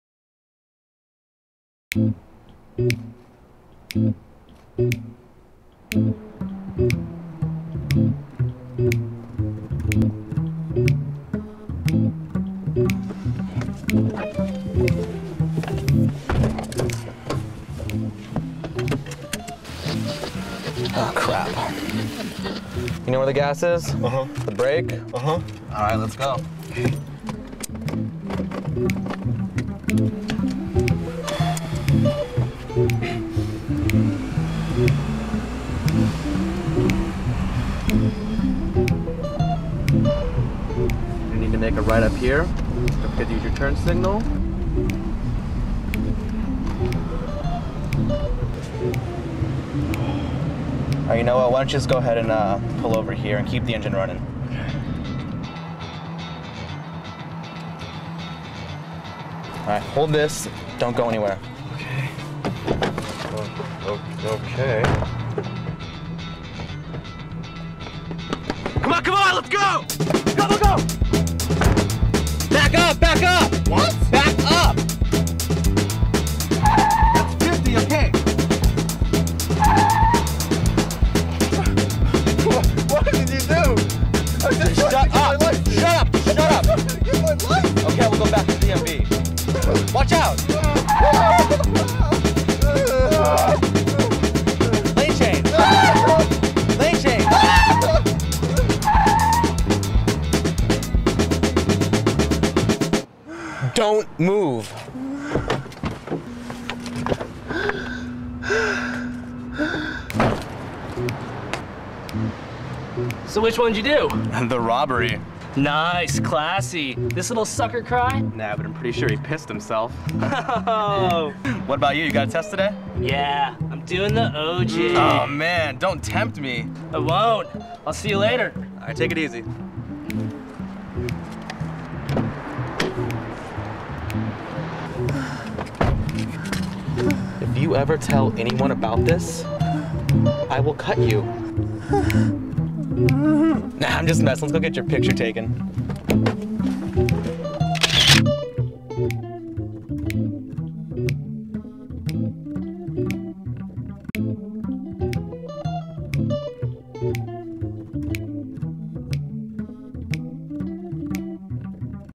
Driver's Dred - Apple 2007 Insomnia Film Festival
24 Hour Apple Insomnia Film Festival.  A boy takes his driving test, which turns out being much more than he expected.